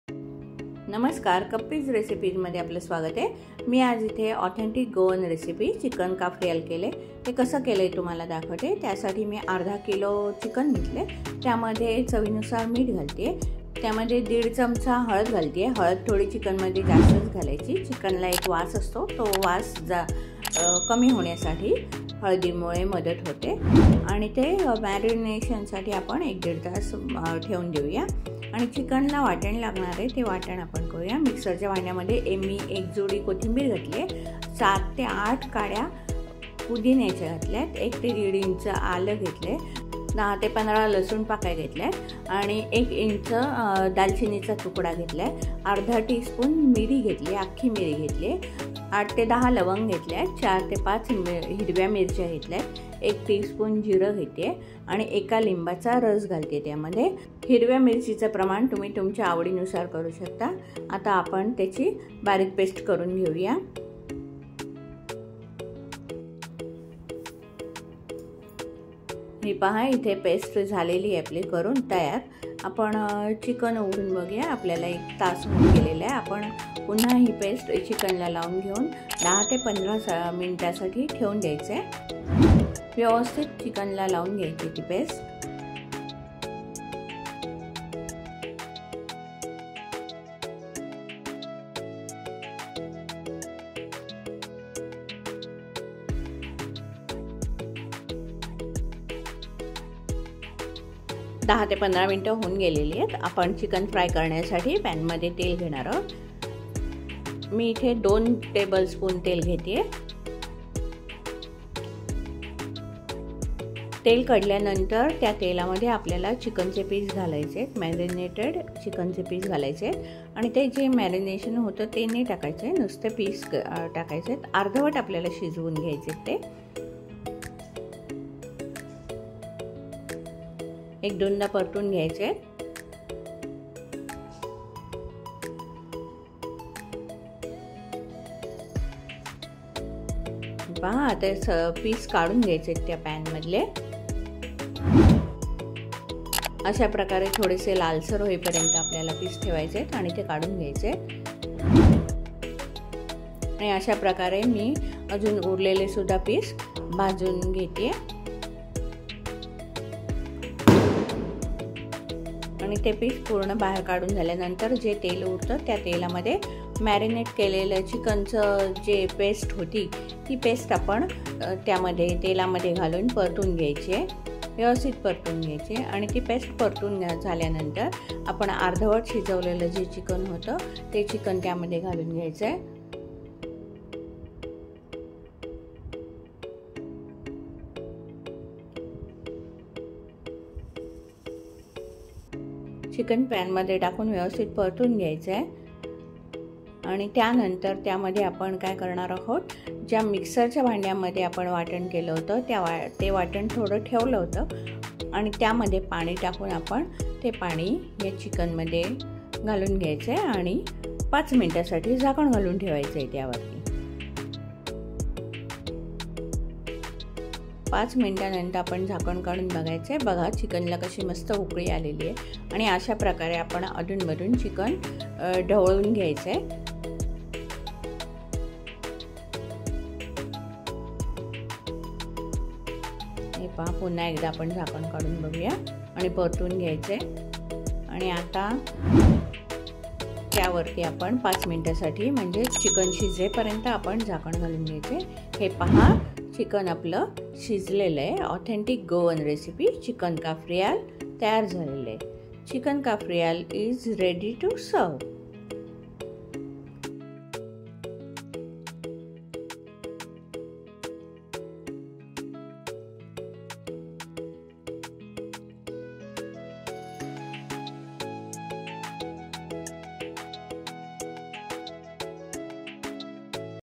Authentic Goan recipe Chicken Cafreal 🍗 | @Kappi'sRecipes
Hi viewers today I am going to present you mouth watering & delicious non veg recipe. Authentic Goan recipe Chicken Cafreal 🍗. I hope you will like this recipe & try this recipe at home .
channel.

Ingredients:-
1) Chicken half kg.
2) Salt to taste
3) Turmeric powder 1 & half teaspoon
4) Fresh Coriander 1 bunch
5) Pudina 7 to sticks
6) Ginger 1 & half inch
7) Garlic cloves 10 to 15
8) cinnamon 1 inch
9) Half teaspoon black pepper
10) Cloves 8 to 10
11) Green chilli 4 to 5
12) Cumin seeds 1 teaspoon
13) Lemon juice 1
14) Oil 2 Tablespoons
15) Lemon & red chilli for garnishing

Your query:-
How to make chicken?
How to make authentic chicken?
How to make chicken Cafreal?
How to make an authentic Goan recipe?